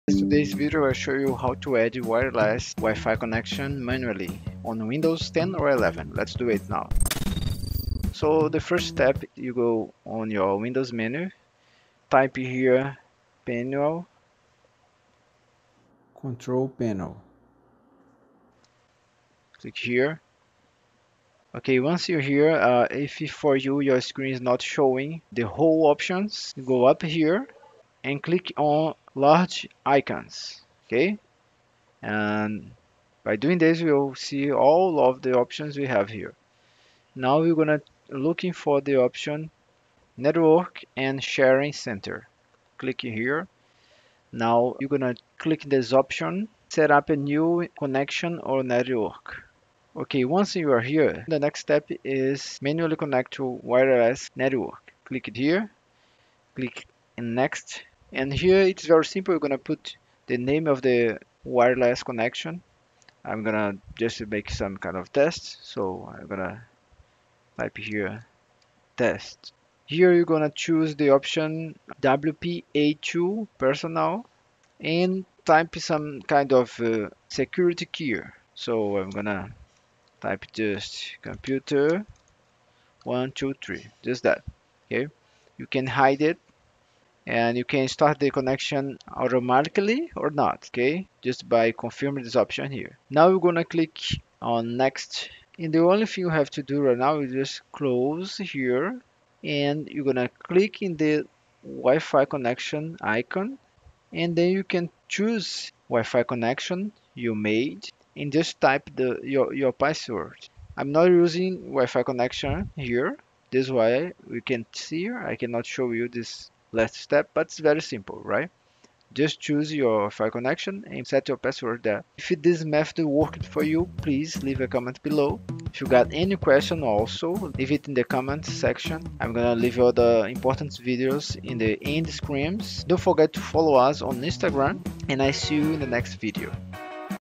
How to Add Wireless Wifi Network Manually on Windows PC or Laptop
Having trouble connecting to a WiFi network that doesn't show up? In this video, I’ll show you how to manually add a wireless network in Windows 10 or 11 step by step. Whether your WiFi network is hidden, not broadcasting the SSID, or your laptop just can’t find the signal, this tutorial will help you connect to a hidden WiFi network manually and fix the issue.

✅ What you’ll learn in this video:
How to manually connect to a WiFi network in Windows
Steps to add a hidden wireless network
How to fix WiFi not showing up on your Windows PC or laptop
Tips for setting up WiFi when the network is not detected

🖥️ This works on both Windows 10 and Windows 11 laptops or desktops.

🔧 If your wireless connection isn’t showing, or your device can’t find your home or office WiFi, this quick fix will get you connected in no time!